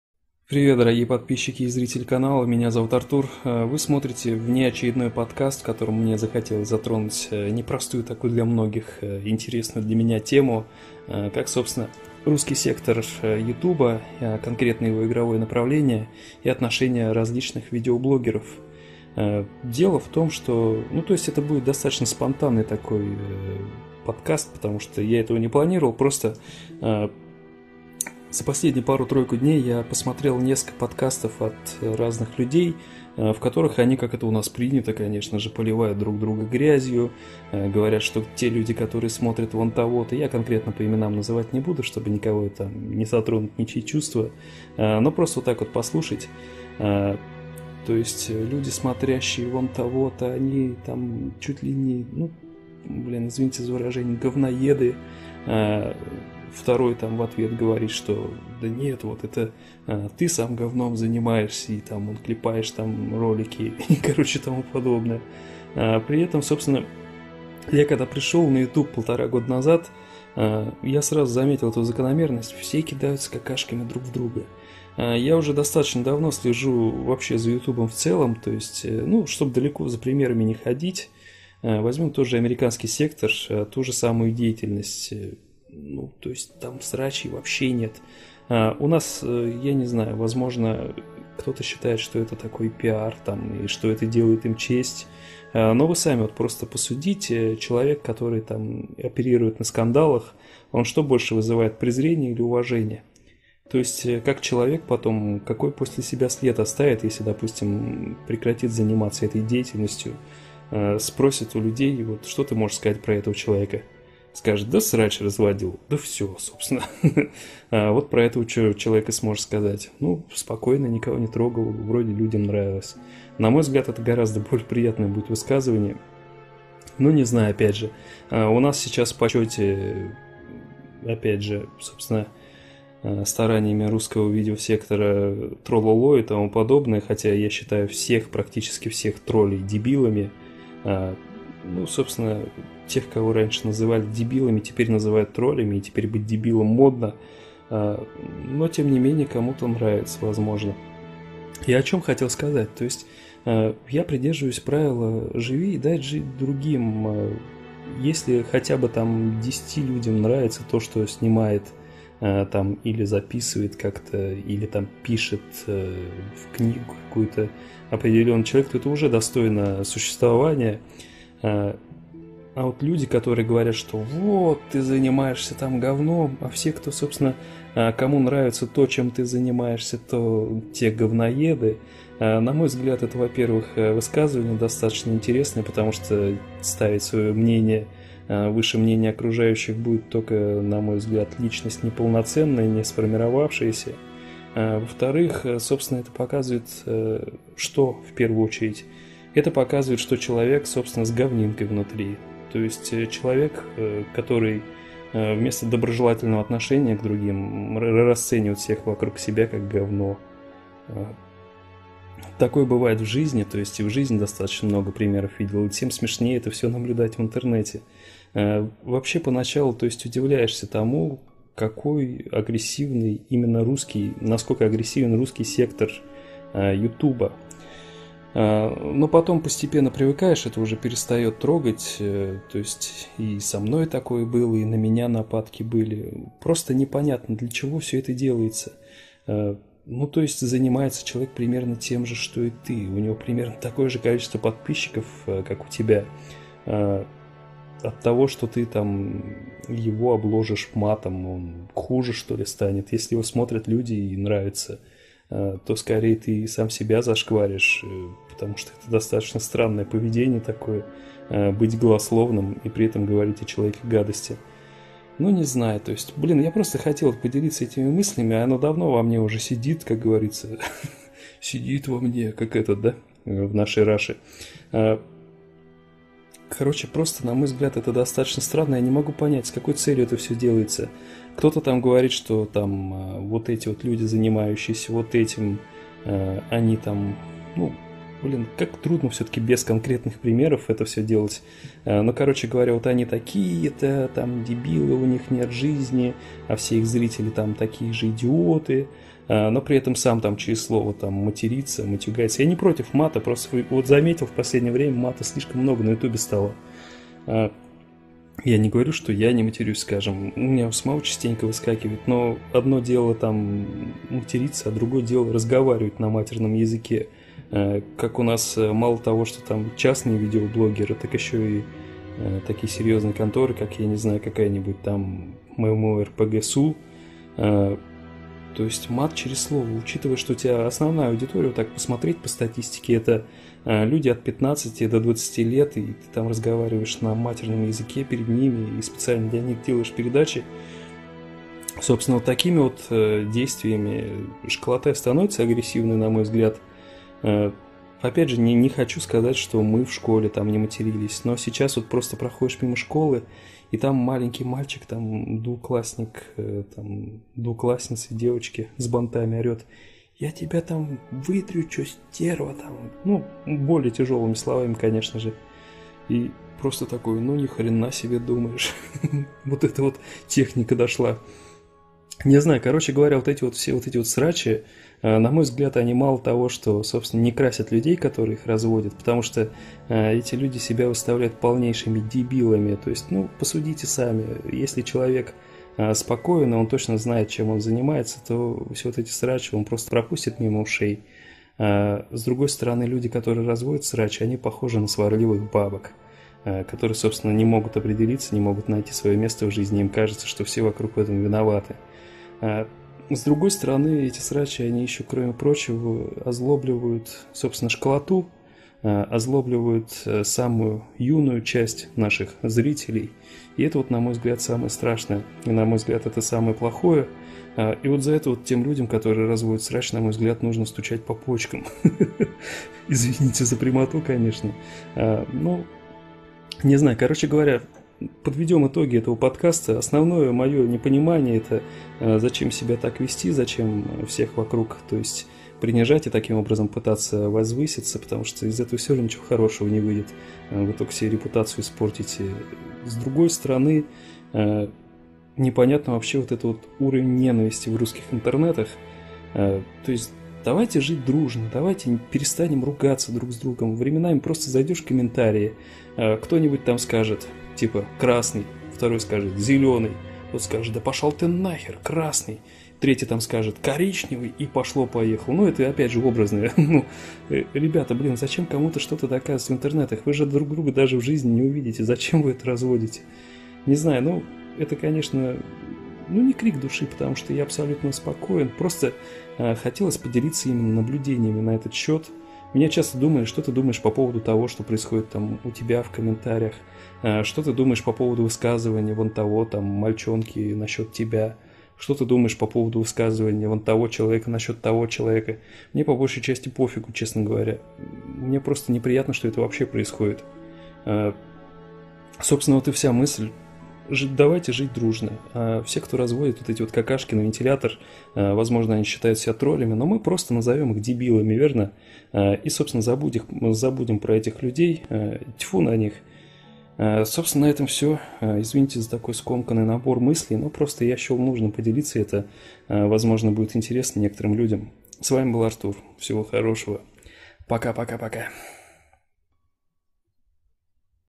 BlackSilverUfa про видеоблоггинг в РФ (2012)
Не помню точный даты подкаста, если есть олдфаги, напишите пожалуйста в комментариях)

P.S. Я Не Артур, я просто перезаливаю его старые прохождения, которые были удалены. Вот причина почему были удалены видео (небольшой подкаст)